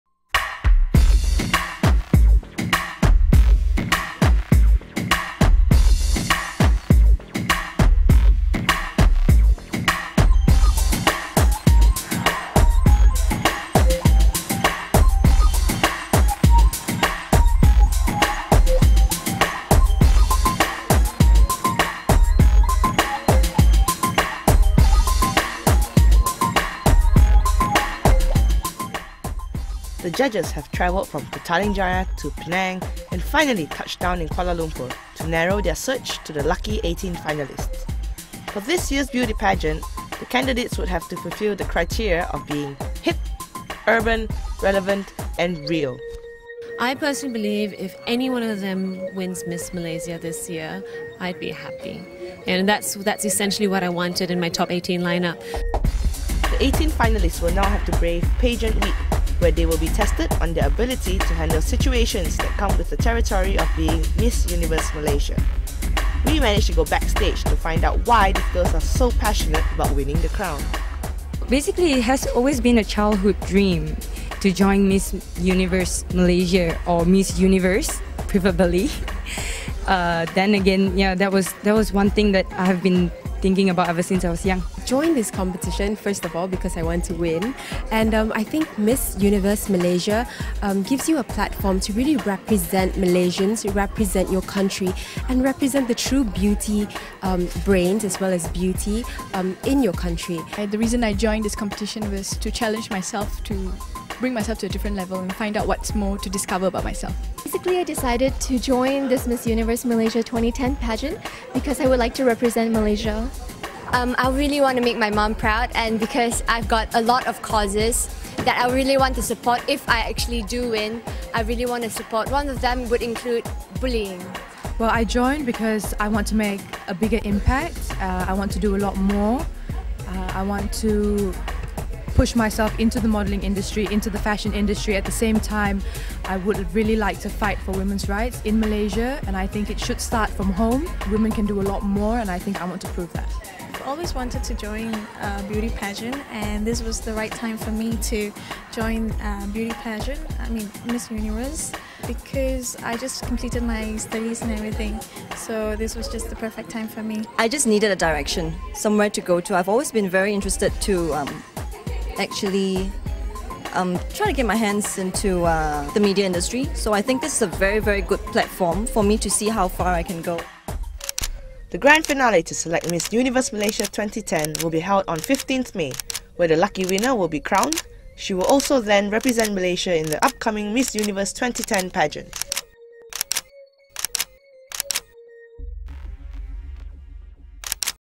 18 beauties revealed
18 girls vying for the title of Miss Universe Malaysia 2010 were unveiled to the media at The Royale Chulan Kuala Lumpur.